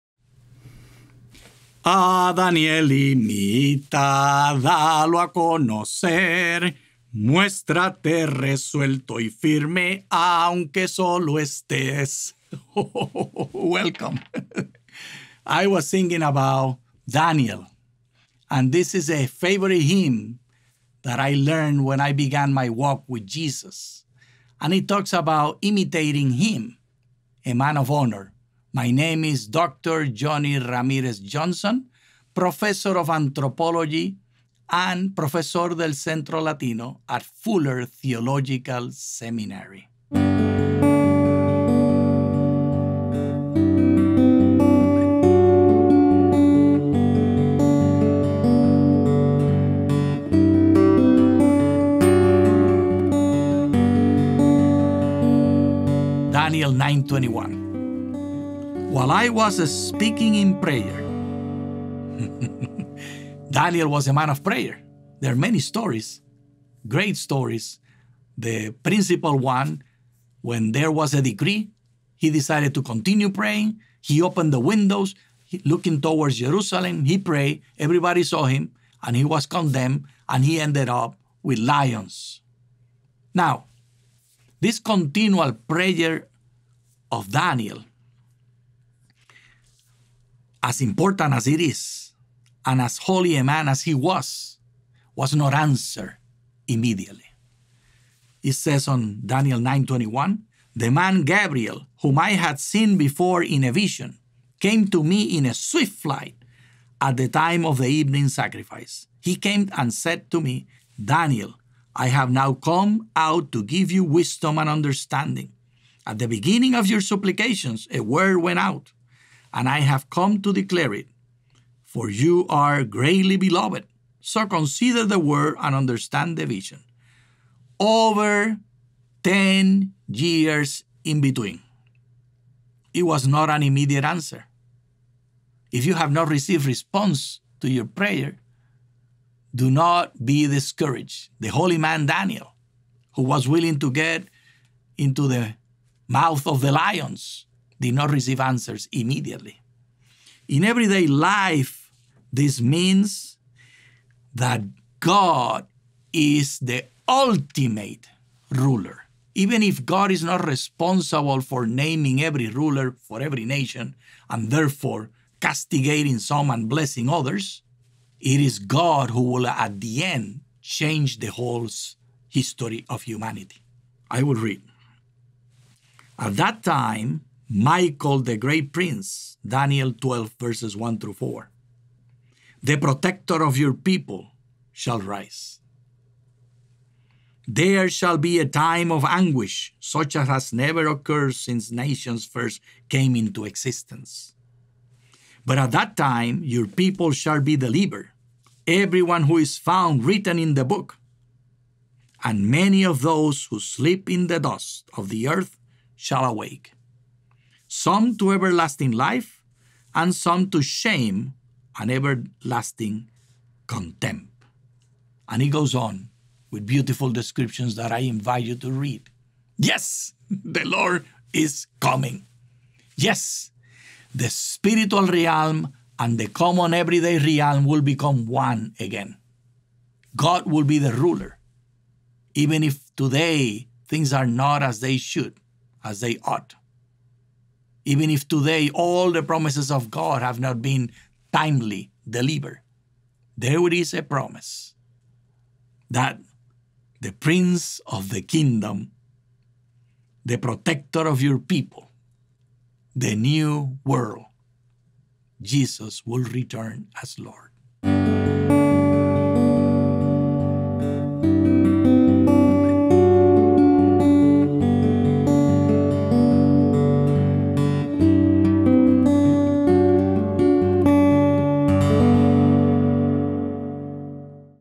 Introduction to Daniel
Dr. Johnny Ramírez-Johnson introduces Daniel by reflecting on how the book shows Daniel’s prayers often going unanswered, yet still portrays God as the ultimate ruler who will have the final say in the history of humanity.